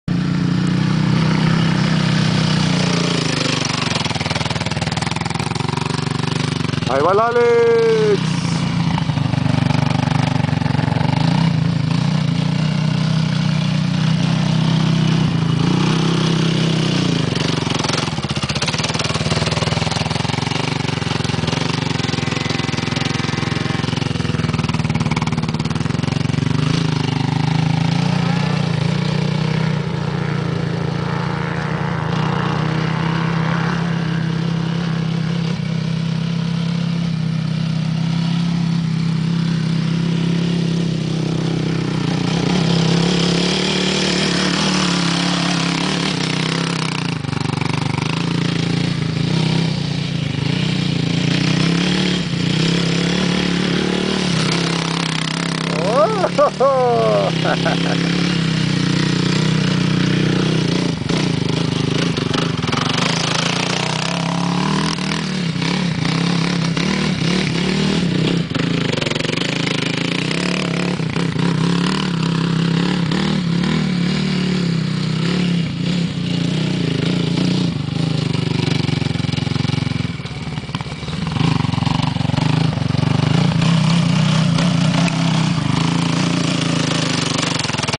Kicking Up Dust
Alex kicking up some dust.